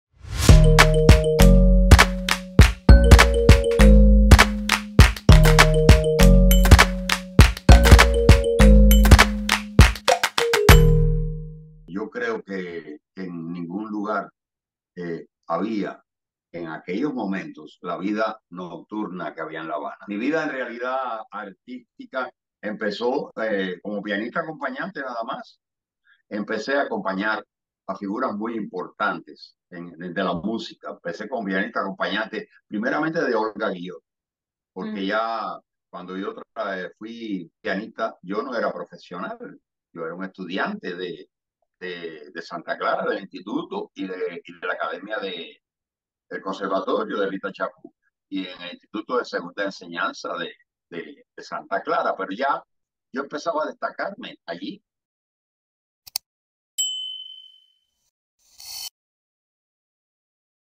Entrevista a Meme Solís en ElZoomDeDDC
Meme Solís: 'Antes de 1959, La Habana era un lugar donde todos querían estar'.

El pianista y cantautor cubano habla con DiarioDeCuba sobre los años de apogeo en su carrera, la creación de la banda 'Los Meme' y su salida del país.

SÍGUENOS:
IG: DIARIO DE CUBA (@diariodecuba)
TikTok: DIARIO DE CUBA's Creator Profile